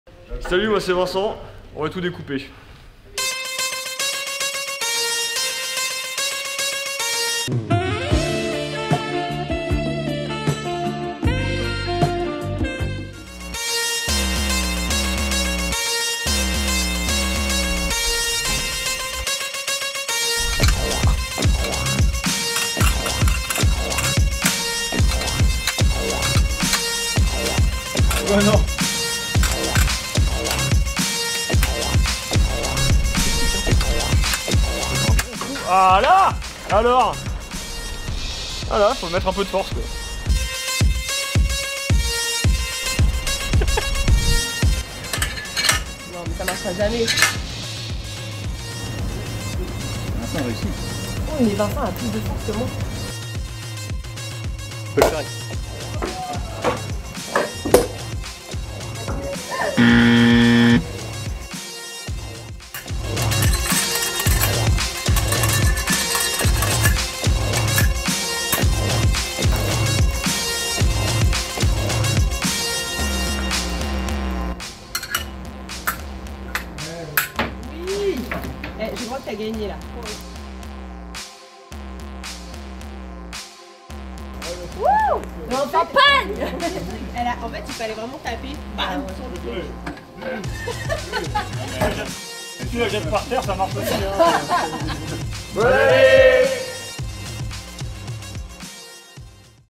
Quatre sabreurs, quatre manières de sabrer le champagne !
Pour les fêtes, voici un petit guide pour sabrer le champagne!
Vincent Anstett, Charlotte Lembach, Boladé Apithy et Manon Brunet vous montrent comment ils font!
A vous de choisir la méthode qui vous convient le mieux !